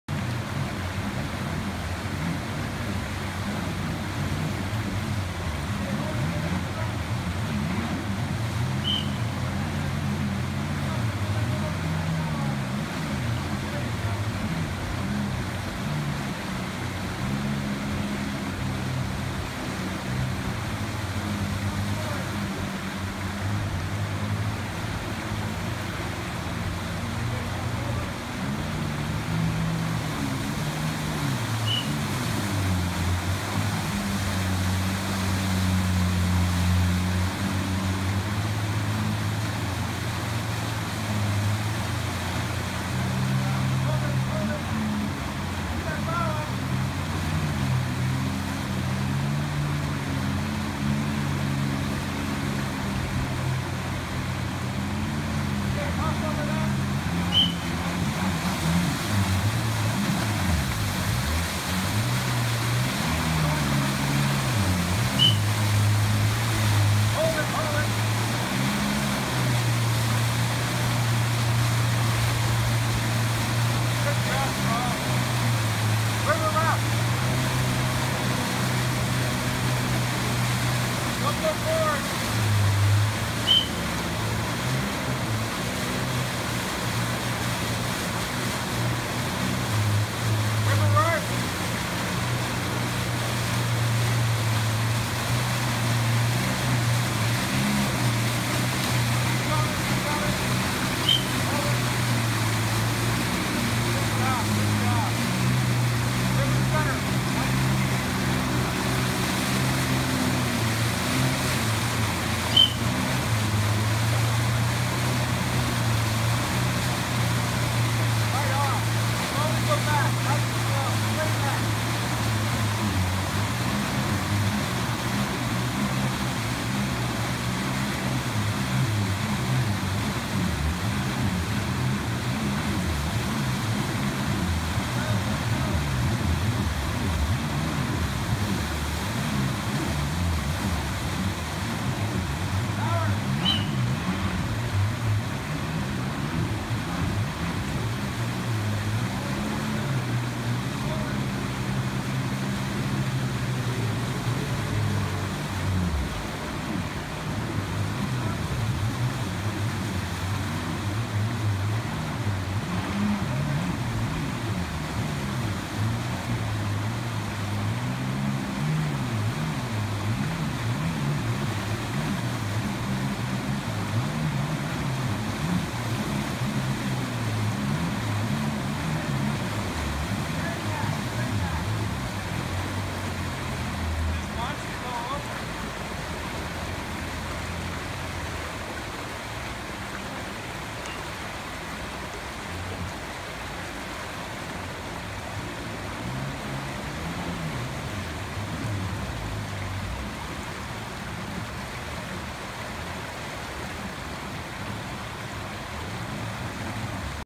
AlumaPro "Control & Finess" Exercise- IAWRP Conference
"Control & Finess" Exercise w/ the AlumaPro at the IAWRP Conference during Tony Hargett's 3 day swiftwater rescue course.